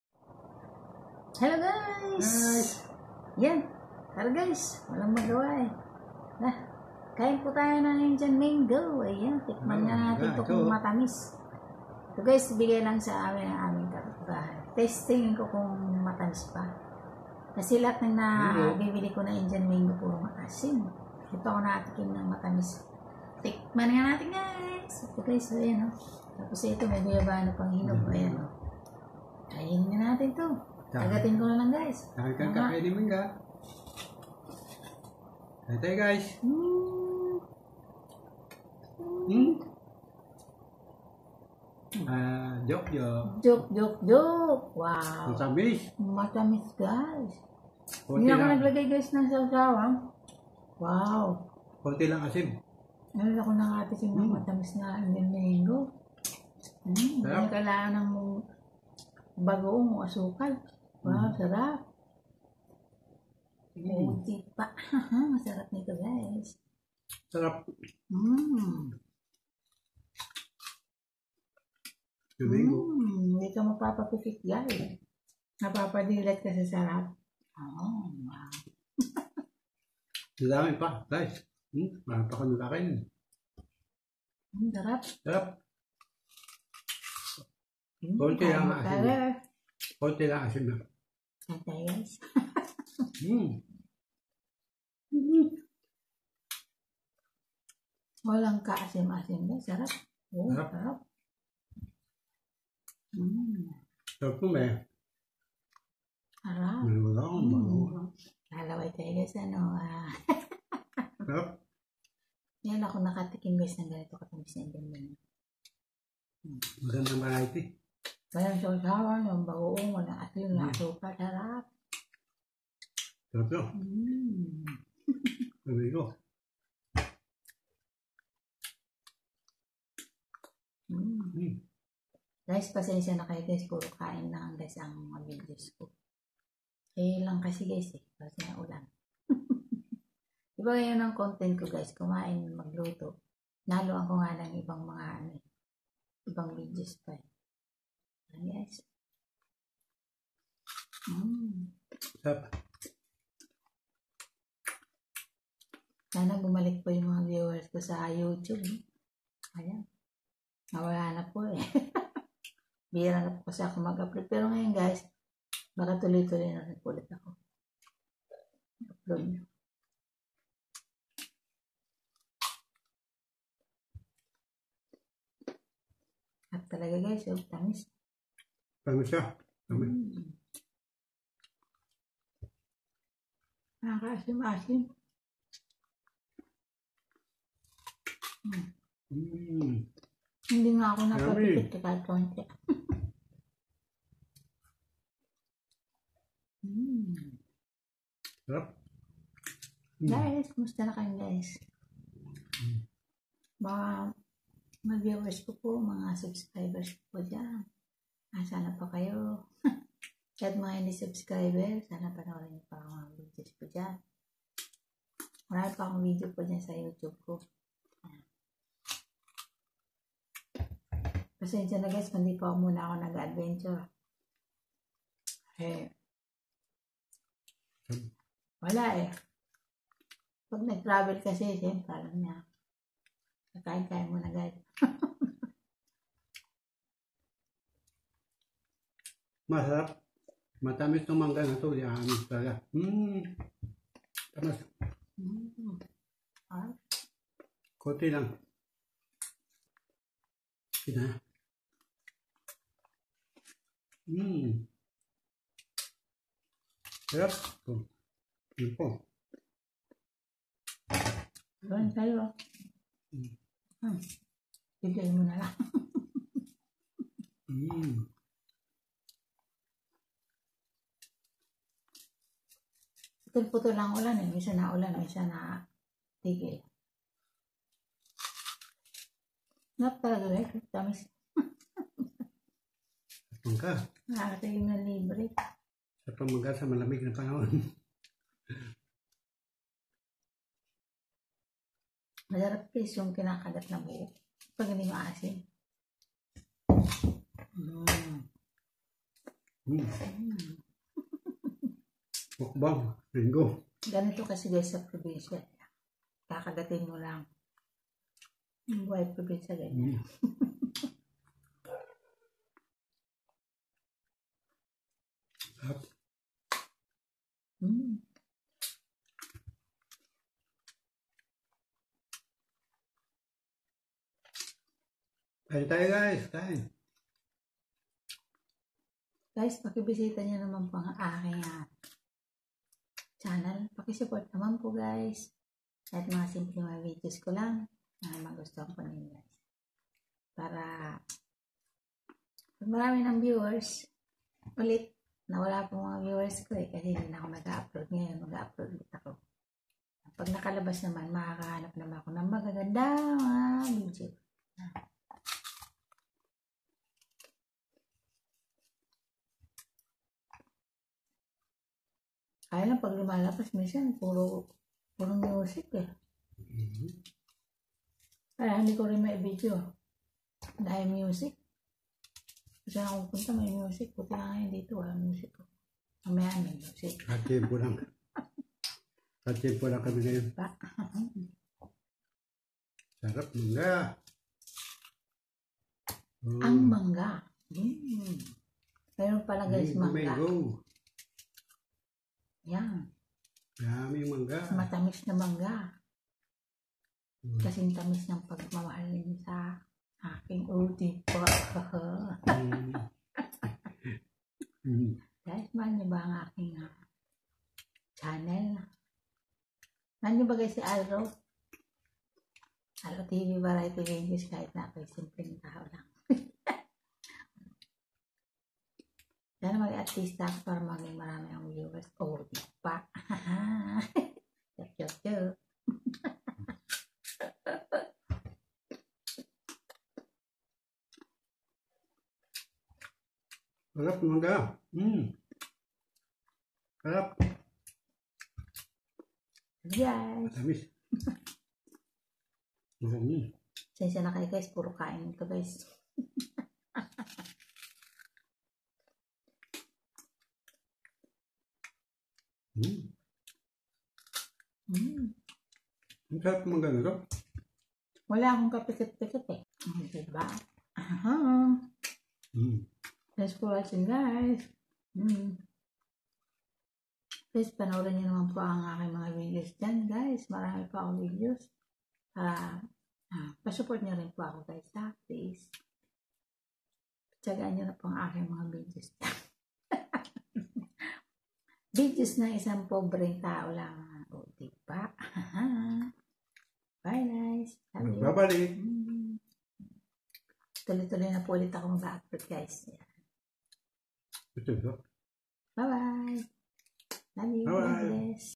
masarap kumain ng mangga @alrotv variety videos
@alrotv variety videos, welcome to my channel,mapagpalang araw po sa inyong lahat,kumusta na po kayong lahat guys,masarap kumain ng mangga,Tara kain po tayo ng Matamis na mangga,thanks for watching po,please likes, comments,keep safe always and God bless us all.